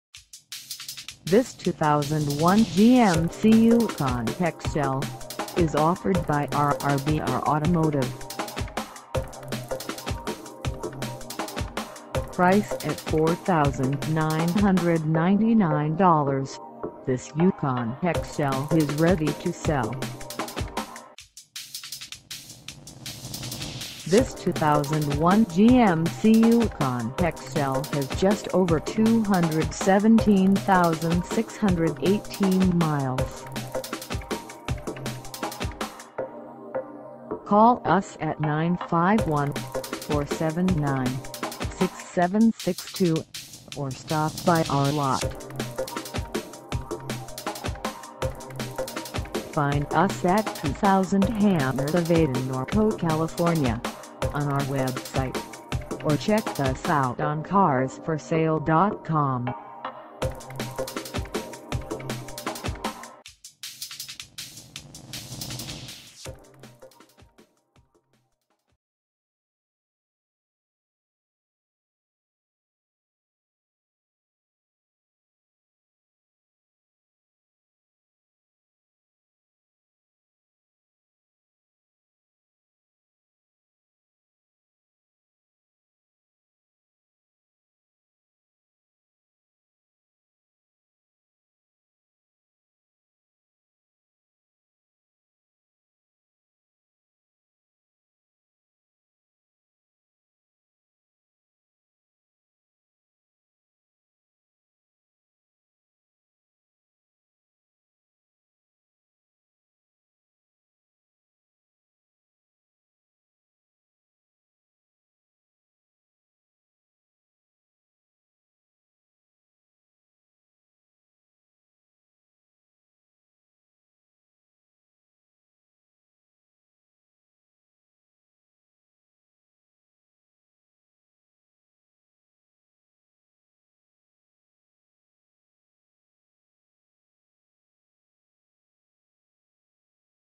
2001 GMC Yukon XL SLE - for sale in NORCO, CA 92860
RRBR Automotive
2000 Hamner Ave

Come test dirve this 2001 GMC Yukon XL SLE for sale in NORCO, CA.

You wont find any electrical problems with this vehicle. This vehicle has no known defects. There are no dings visible on the exterior of this vehicle. The engine is mechanically sound and can be driven anywhere with confidence. Extra clean exterior. This vehicle was kept in the garage. The previous owner was a non-smoker. Extra clean interior. Absolutely no rips, odors or abuse. The mileage represented on this vehicle is accurate. The transmission in this vehicle still shifts smoothly. These tires have about half their life span left.